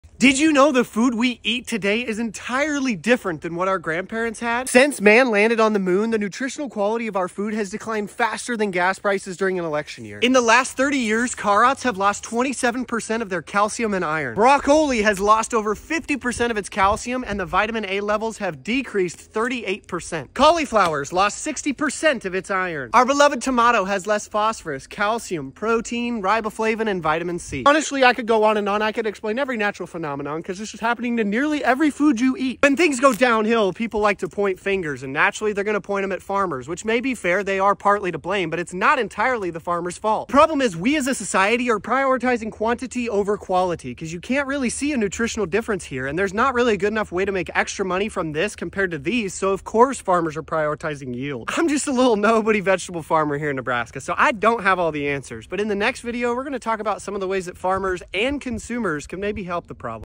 Your food is NOT as nutritious! 🥦
Things are getting a little too chummy around here, so I thought it was time to ruffle some feathers with the truth bomb about your food. It’s NOT the same food your grandparents ate.

According to USDA research, to get the same nutritional value from a carrot today as you would have from a carrot in the 1970s, you would need to eat 1.5 times more carrots. Broccoli has lost over 50% of its calcium. Spinach has 30% less iron. Tomatoes have less protein, riboflavin, phosphorus, calcium and vitamin c. The list goes on and on.

Why? It’s not just on farmers. It’s also about consumer demand and the way we’ve prioritized cheap, abundant food over nutrient-dense crops. Farmers don’t see a market for better produce because you can’t see nutrient quality, and even if you could, it’s rarely cost-effective. For most farmers to stay afloat, they have to focus on yields over quality.

I am but a lowly Nebraska produce farmer, so I don’t have all the answers, but we’re gonna discuss some of the solutions in the next video!